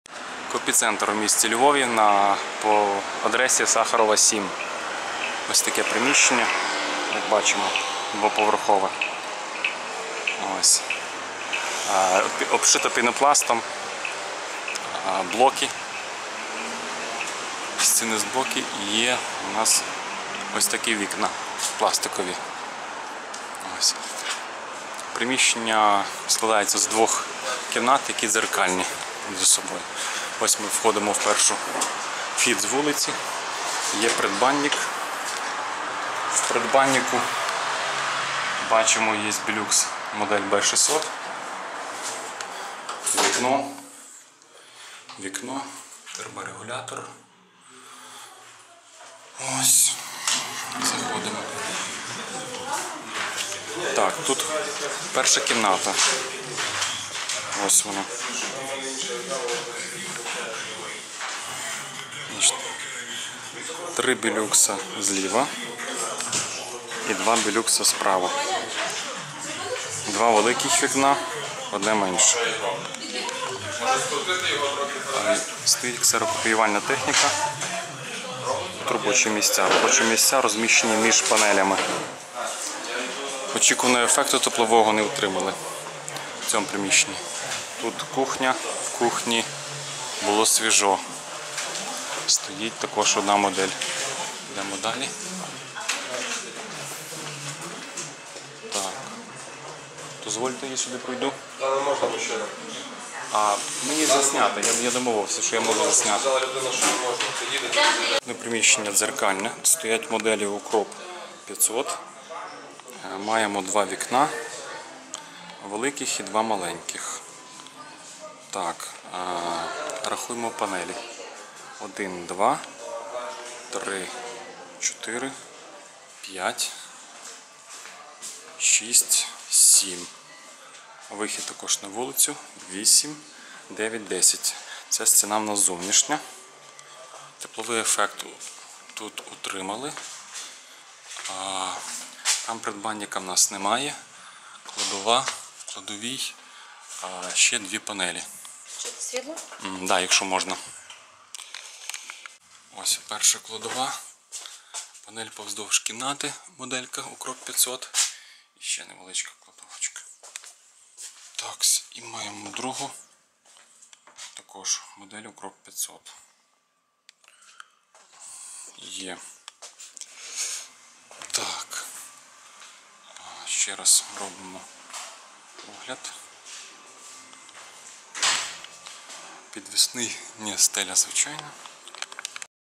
Отзыв. Отопление магазина-офиса инфракрасными панелями Укроп и Билюкс. Копицентр, Львов
Честный отзыв на отопление офиса копицентра инфракрасными обогревателями: настенными панелями УКРОП и потолочными панелями Билюкс.
Установлено индивидуальную систему электроотопления офиса инфракрасными длинноволновыми панелями с настенными механическими терморегуляторами.
1этаж 2этажного здания, 2 офиса по 60кв.м.
Адрес: г.Львов, Украина.
Задача: комфорт, экономия в сравнении с газом и другими электроотоплением.

Результат: тепло, но не совсем экономно, зимой в мороз ср.суточное потребление 160кВт.
Отзыв на ик-панели УКРОП: комфорт -хорошо, экономия -ниже ожидаемого.
Отзыв на ик-панели Билюкс: комфорт -плохо, экономия -ниже ожидаемого.

-магазин эко-обогревателей -альтернативное отопление без газа.